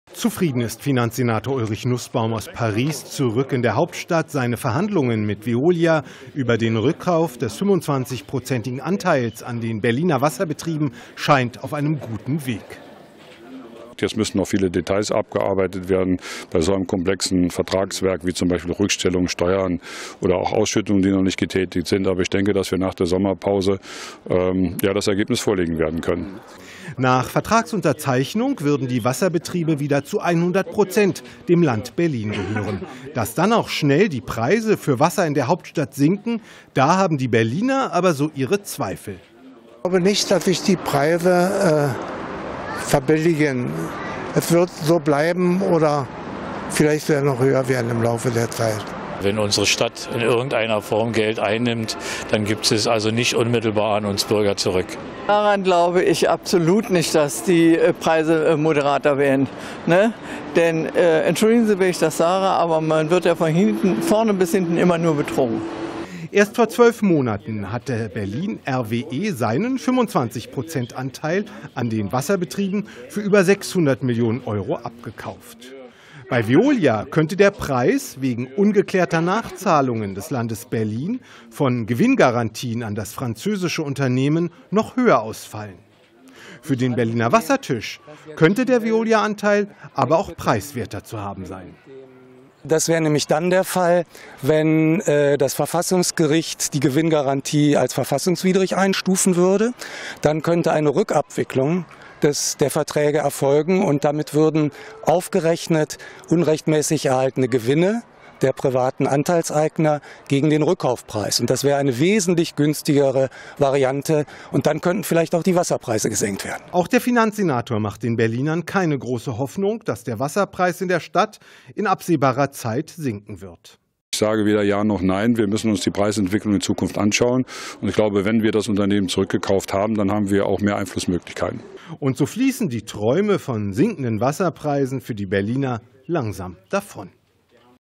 Übernahme der Berliner Wasserbetriebe ist auf gutem Weg !
Die Verhandlungen mit Veolia ,über den Rückkauf des 25 Prozentigen Anteils an den Berliner Wasserbetrieben, scheinen auf einem guten Weg. Nach Vertragsunterzeichnung würden die Wasserbetriebe wieder zu 100 Prozent der dem Land Berlin gehören. Für Veolia könnte der Erlös - wegen ungeklärter Nachzahlungen des Landes Berlin von Gewinngarantien an das Unternehmen - höher ausfallen.